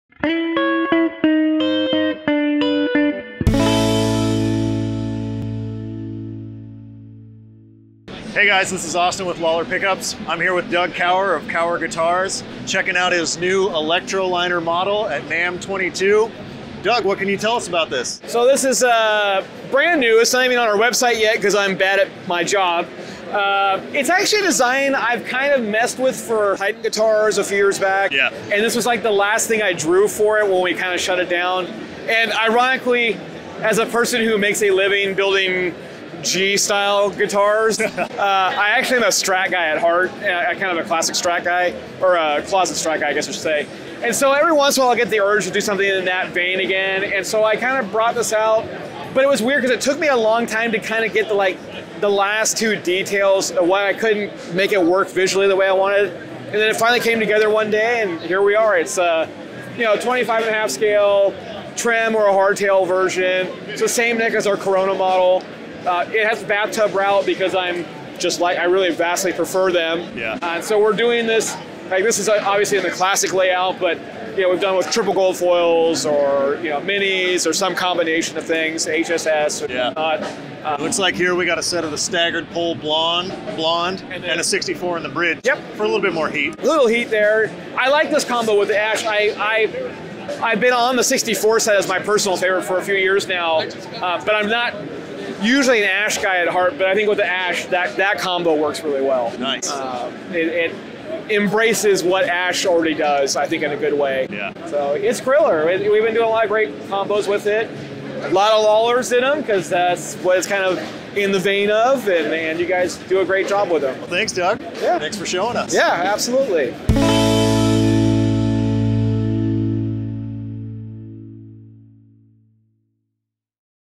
LOLLAR PICKUPS - NAMM Show 2022 - Kauer Guitars
Lollar Pickups' own Austin Patterson talks with Doug from Kauer Guitars about the new Electroliner model guitar featuring Lollar Blonde and Sixty-Four Strat pickups.